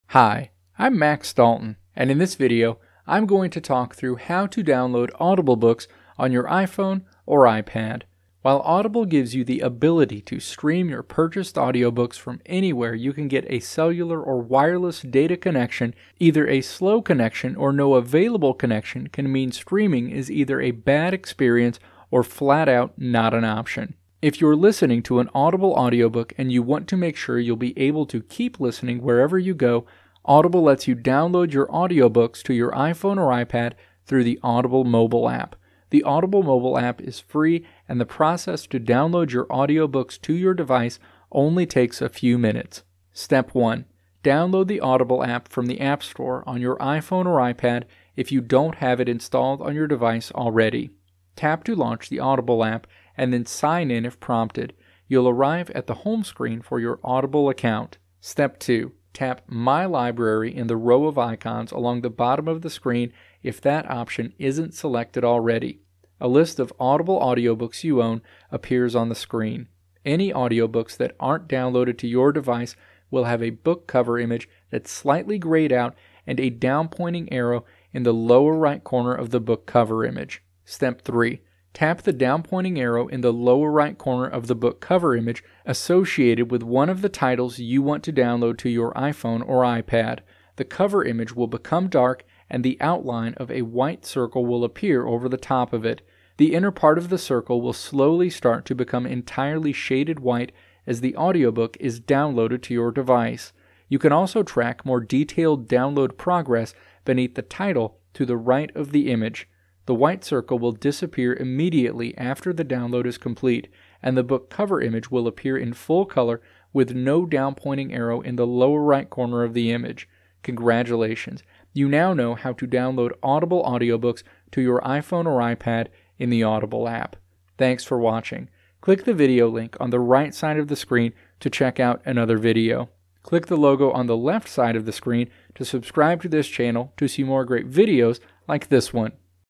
How to Download Audible Books on iPhone or iPad
This video walks you through how to download Audible audiobooks to your Apple iPhone or iPad.

Video Transcript:
While Audible gives you the ability to stream your purchased audiobooks from anywhere you can get a cellular or wireless data connection, either a slow connection or no available connection can mean streaming is either a bad experience or flat-out not an option. If you're listening to an Audible audiobook and you want to make sure you'll be able to keep listening wherever you go, Audible lets you download your audiobooks to your iPhone or iPad through the Audible mobile app. The Audible mobile app is free, and the process to download your audiobooks to your device only takes a few minutes.

Step 1. Download the Audible app from the App Store on your iPhone or iPad if you don't have it installed on your device already. Tap to launch the Audible app, and then sign in if prompted. You'll arrive at the home screen for your Audible account.

Step 2. Tap "My Library" in the row of icons along the bottom of the screen if that option isn't selected already. A list of Audible audiobooks you own appears on the screen. Any audiobooks that aren't downloaded to your device will have a book cover image that's slightly grayed out and a down-pointing arrow in the lower right corner of the book cover image.

Step 3. Tap the down-pointing arrow in the lower right corner of the book cover image associated with one of the title's you want to download to your iPhone or iPad. The cover image will become dark, and the outline of a white circle will appear over the top of it. The inner part of the circle will slowly start to become entirely shaded white as the audiobook is downloaded to your device. You can also track more detailed download progress beneath the title to the right of the image. The white circle will disappear immediately after the download is complete, and the book cover image will appear in full color with no down-pointing arrow in the lower right corner of the image.